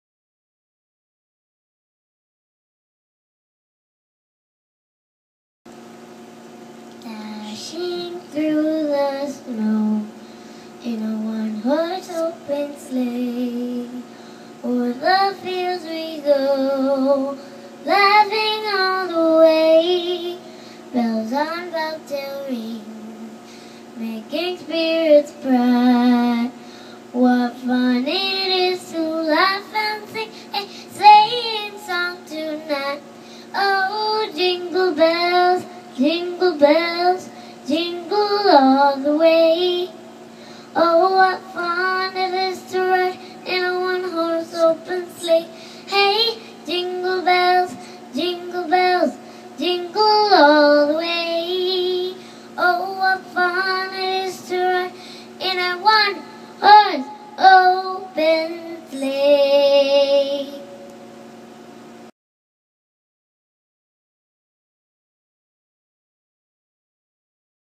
Elizabeth School of Music
This is a wonderful performance from one of our vocal students.